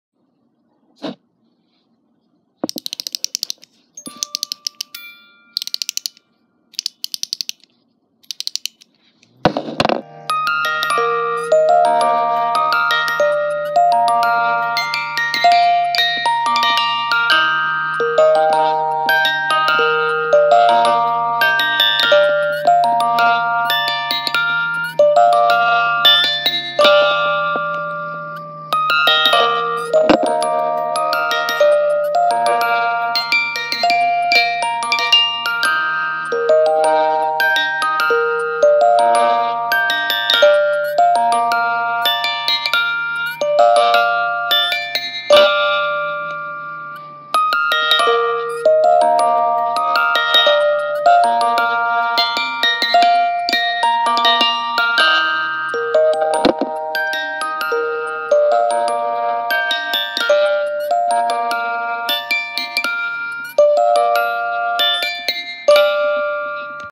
Sankyo music box unknown tune, HELP!
I’m trying to figure out what song it is, HELP!